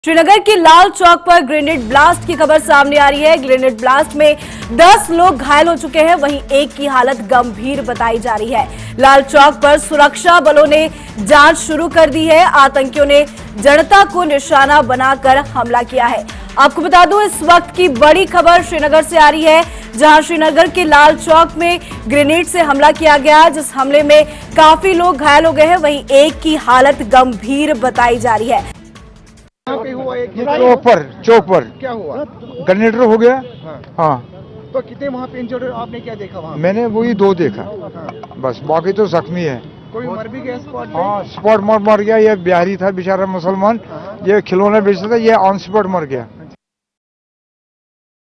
BreakingNews : Shrinagar के लाल चौक पर ग्रेनेड ब्लास्ट, 10 लोग घायल
श्रीनगर के लाल चौक पर ग्रेनेड ब्लास्ट….ग्रेनेड ब्लास्ट में 10 लोग घायल,1 की हालत गंभीर….लाल चौक पर सुरक्षा बलों ने जांच शुरू की….आतंकियों ने जनता को निशाना बनाकर हमला किया